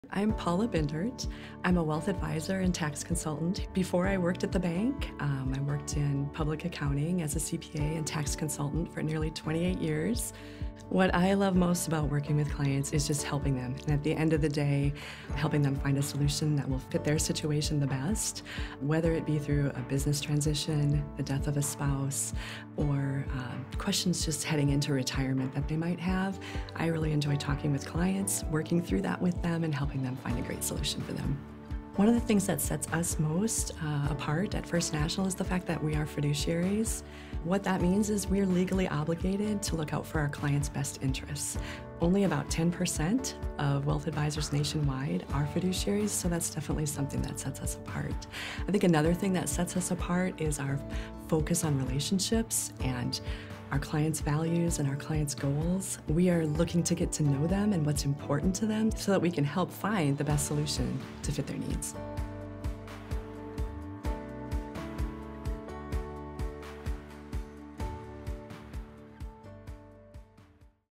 Meet Paula Bindert — Wealth Advisor & Tax Consultant at First National Wealth Management
With over 30 years of combined public accounting, tax consulting, and financial planning experience, Paula Bindert offers unmatched expertise for our wealth management clients.